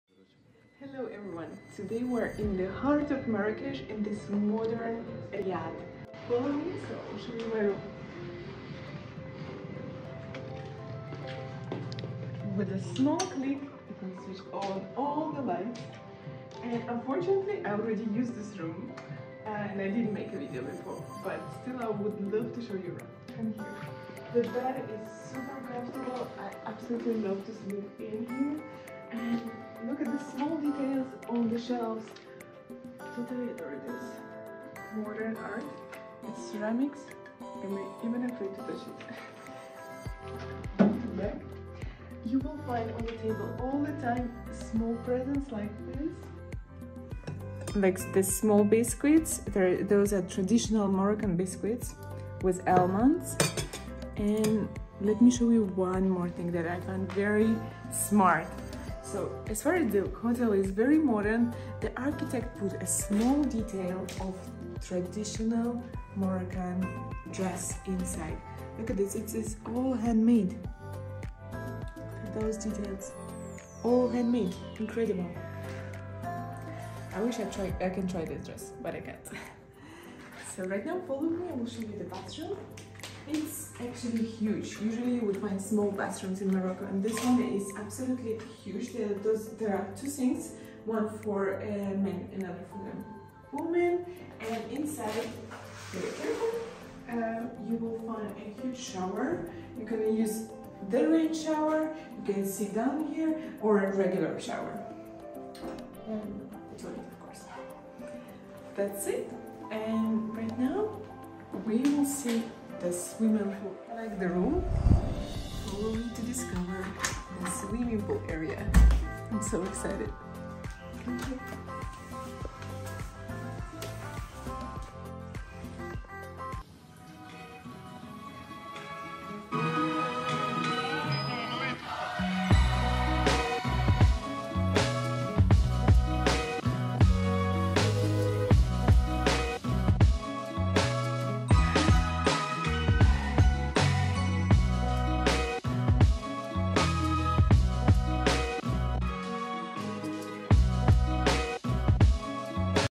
MARRAKECH | Riad in Medina room tour
A dreamy Riad boutique hotel in MARRAKECH Medina @labrillantemarrakech-unehi7746 room tour